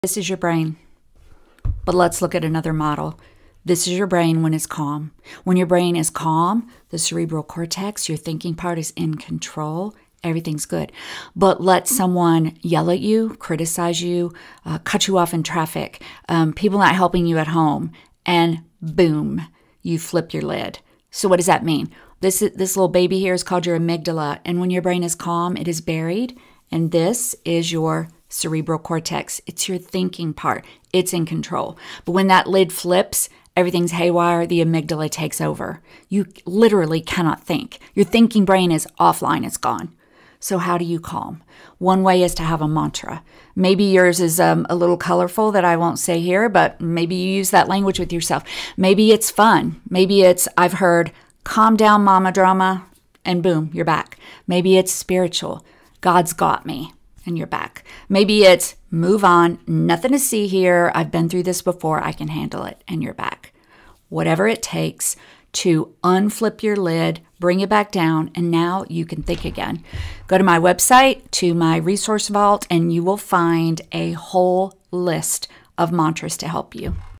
This is your brain. This is your brain on adrenaline.
Ever heard of the hand model of the brain? It's a simple, powerful way to understand why you "lose it" sometimes—and what to do next.
Picture this: someone yells at you. You get upset. BAM—your brain flips its lid. That emotional little part called the amygdala hijacks the whole system. The logical part (your cerebral cortex)? Totally offline.
So how do we get back to thinking clearly?

Start with a calming phrase—your calming phrase.
For some, it’s “Calm down, Mama Drama 🤣.”
For others, it’s... well... more colorful.

Whatever works—use it. That phrase helps your brain settle so you can think again.

Don’t beat yourself up for flipping your lid. Learn how to flip it back.